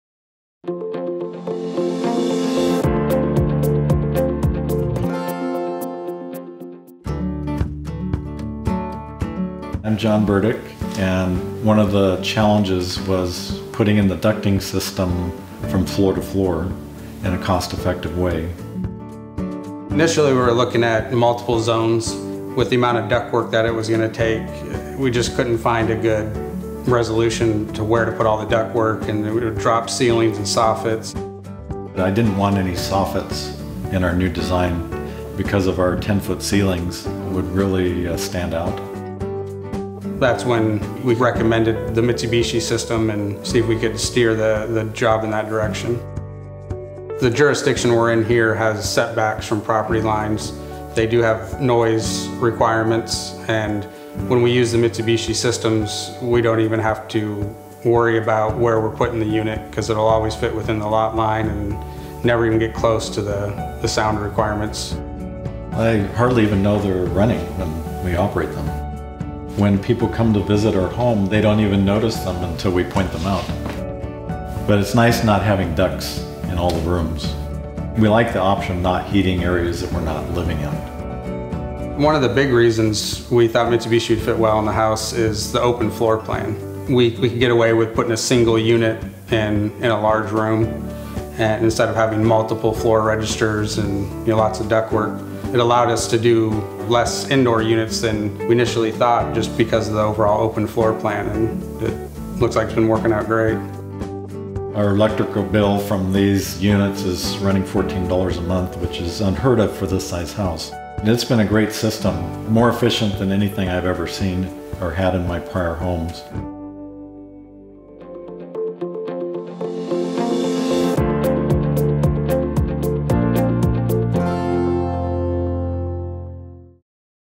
Custom Home Installation (Seattle, WA)
Kliemann Brothers Heating installed our products in this 3-story home with an open floor plan and a view of the Seattle waterfront. The homeowner's resulting electric bills from operating our cooling & heating units? $14 a month.